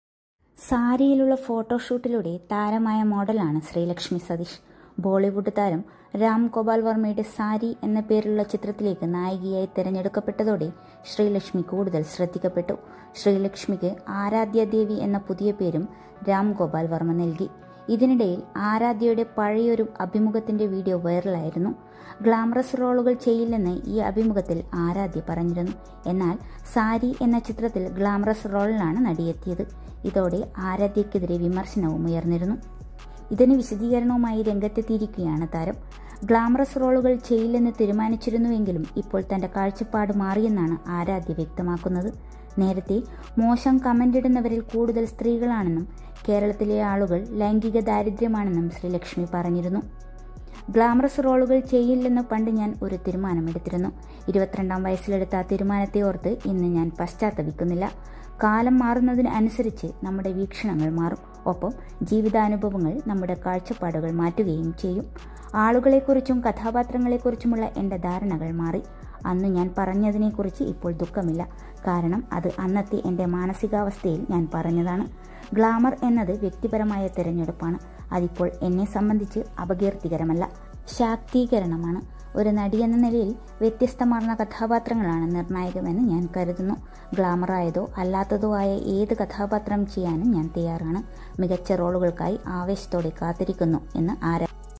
ഗ്ലാമര്‍ വേഷം വ്യക്തിപരം, എന്തിനും തയ്യാർ, അനുഭവം എന്റെ കാഴ്ച്ചപ്പാട് മാറ്റി’; ആരാധ്യ. AaradhyaDevi
ഗ്ലാമറായ വേഷം ചെയ്യാന്‍ തയ്യാറാണ്, കേരളത്തില‍െ ആളുകള്‍ക്ക് ലൈംഗിക ദാരിദ്ര്യം